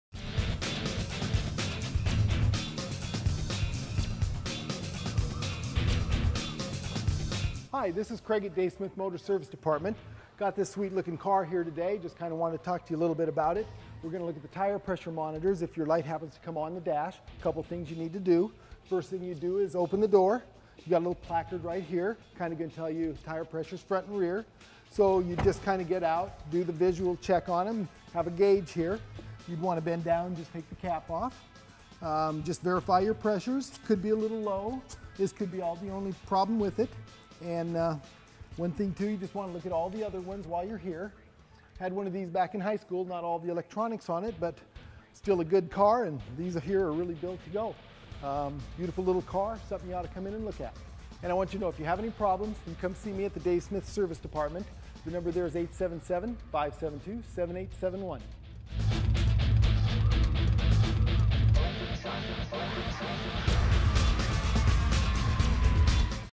Dave Smith Motors Service Tips: Tire Pressure Monitor
Dave Smith Motors wants to ensure your new vehicle lasts as long as possible. So, we've made these videos to help you maintain your new vehicle properly. This video will help you understand what the "TPM" Tire Pressure Monitor is and how to check it yourself.

World's Largest Chrysler Dodge Jeep Ram Dealer

Northwest's Largest GM Dealer